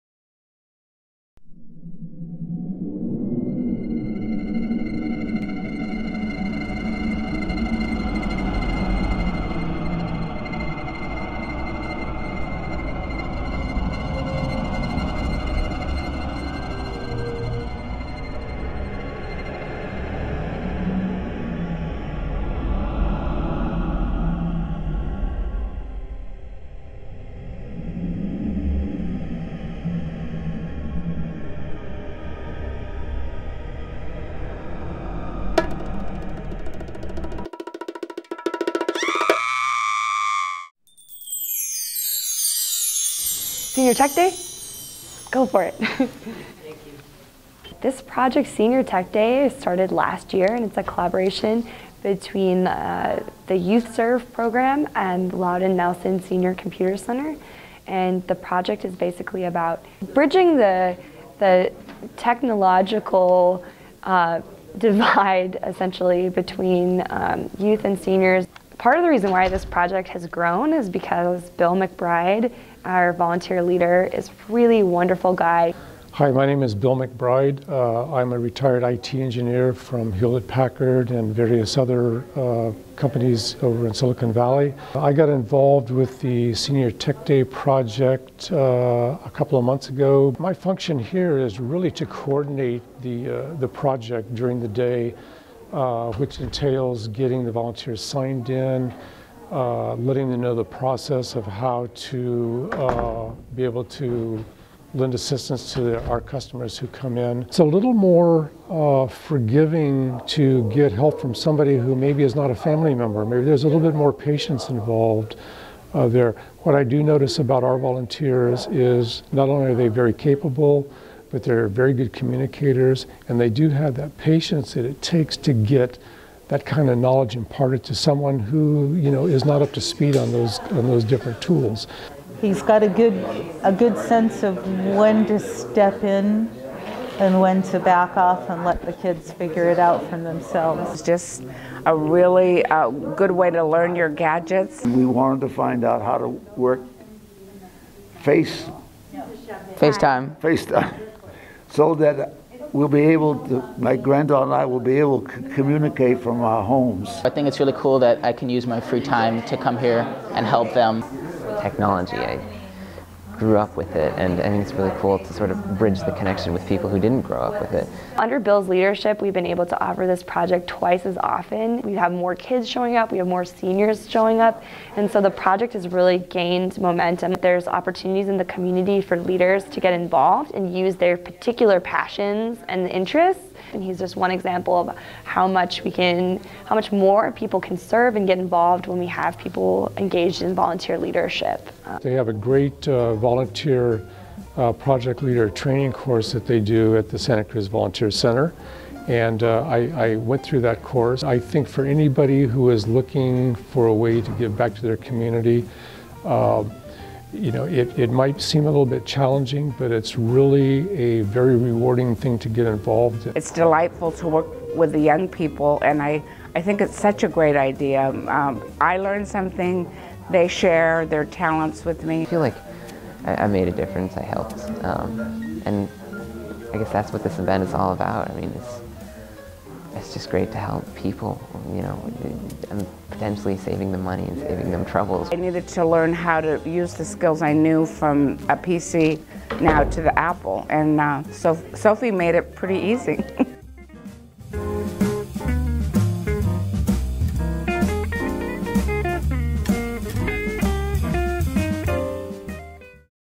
Senior Tech Day
Senior citizens, teens and community Leaders unite for shared technology learning- YouthSERVE, a program of the Volunteer Center of Santa Cruz County and the Senior Computing Center at Louden Nelson Community Center have joined forces to help local seniors navigate technology and social media in the digital age. As "digital natives," young people are the perfect candidates for helping seniors learn how to connect with online technology.

Video created by Barbara Gerry.